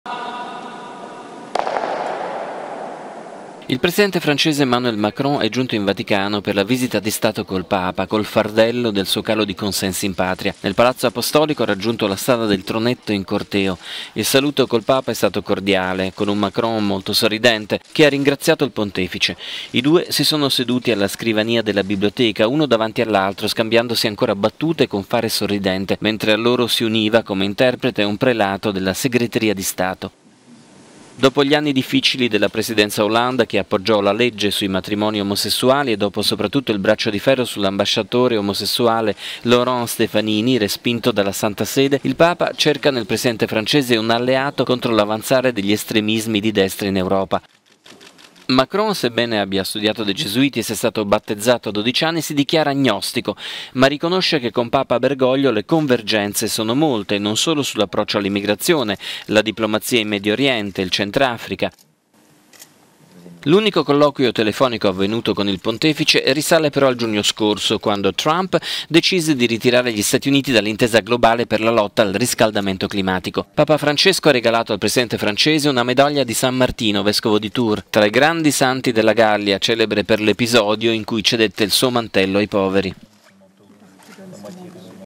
Alta sintonia fra il Papa e il Presidente francese Macron
In visita di Stato in Vaticano Macron ha sfoggiato tutta la sua sintonia col Papa. Grandi sorrisi e un colloquio durato 57 minuti. Un record…